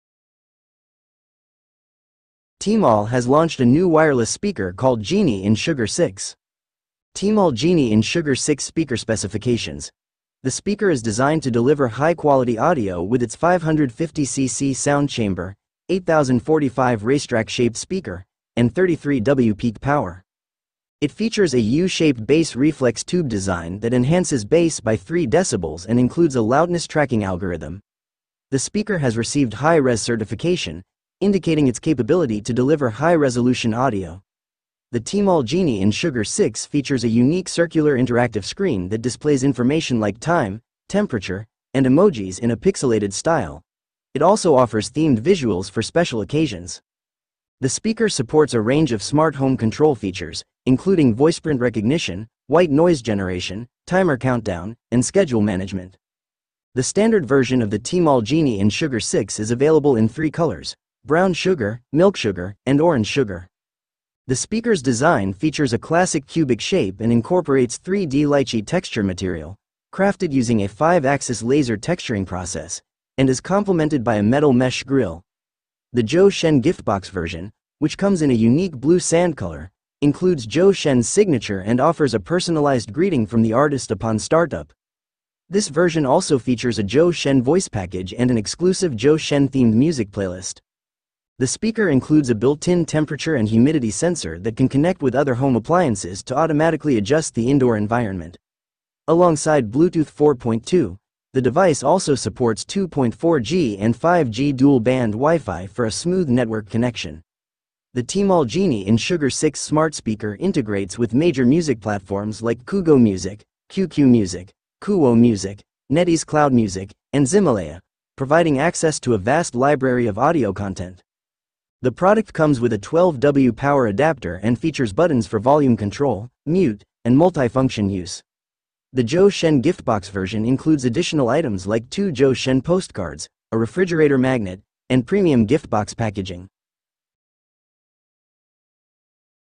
Tmall Genie IN Sugar 6 Speaker : First Look - Review Full Specifications
Tmall has launched a new wireless speaker called Genie IN Sugar 6 in the Chinese market. The smart speaker is priced at 179 yuan ($25) and it is now available on JD.com. The speaker is also available in a special Zhou Shen gift box edition, priced at 249 yuan ($35). Let’s delve into its key specs.